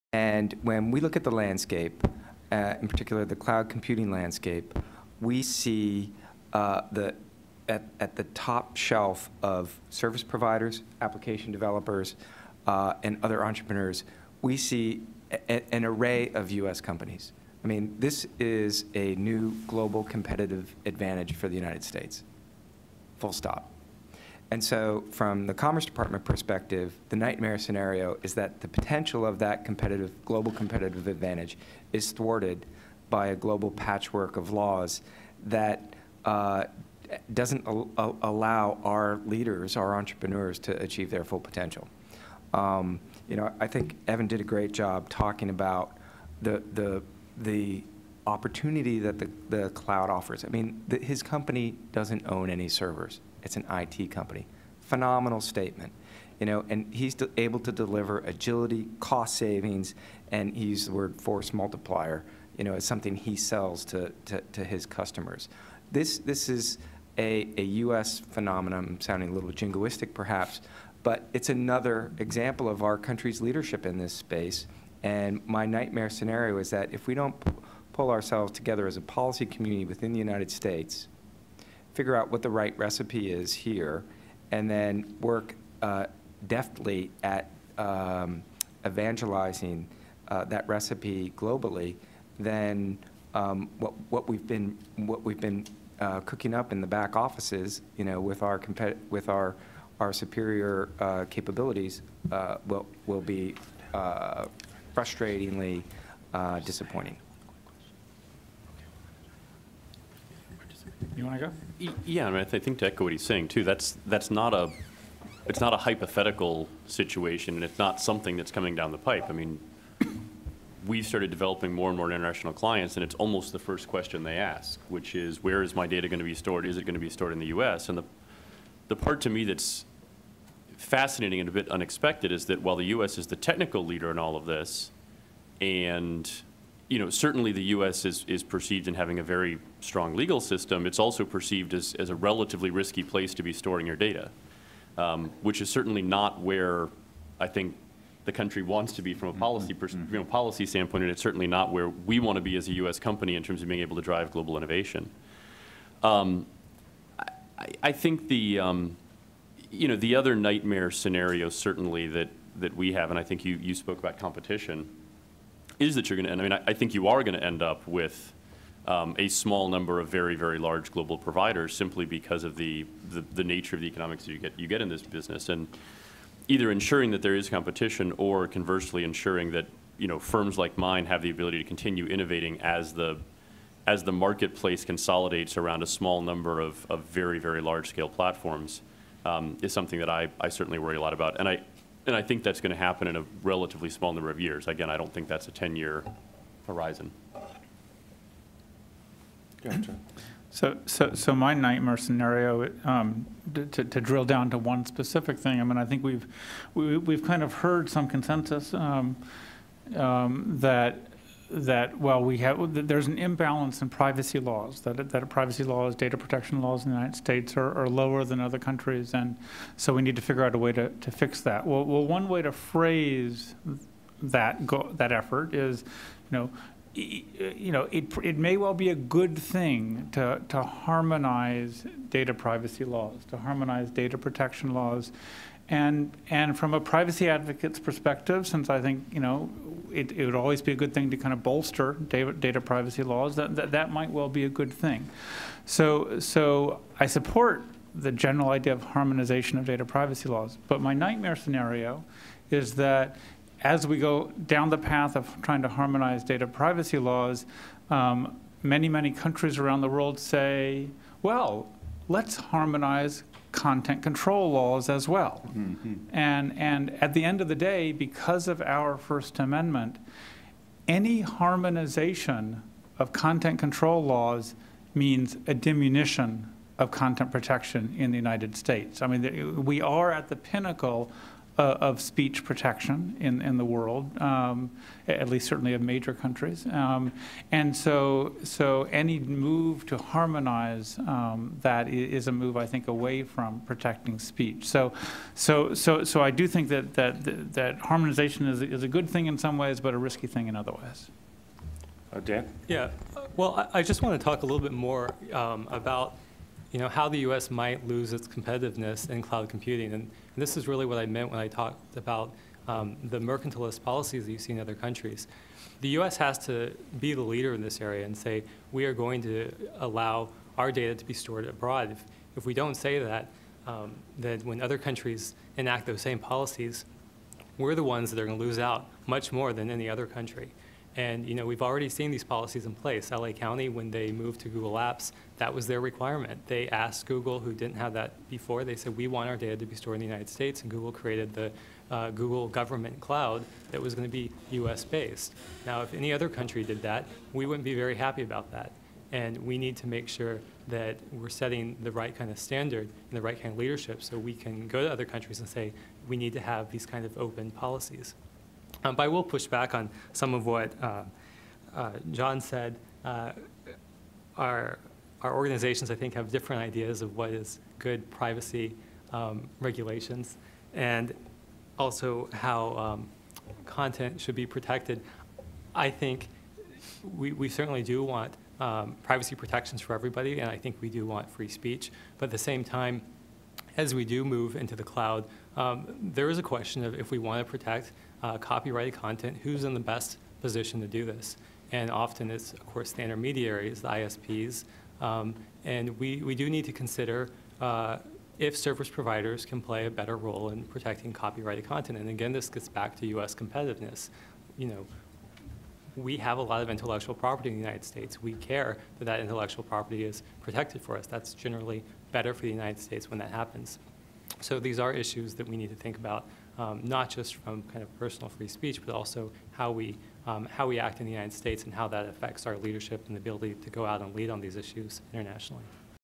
IGF USA, 2010: Worst case scenarios
Panelists Berejka, Evan Burfield, Morris and Castro share potential "nightmare scenarios" and describes hope for harmonizing data policy and continued competition among providers.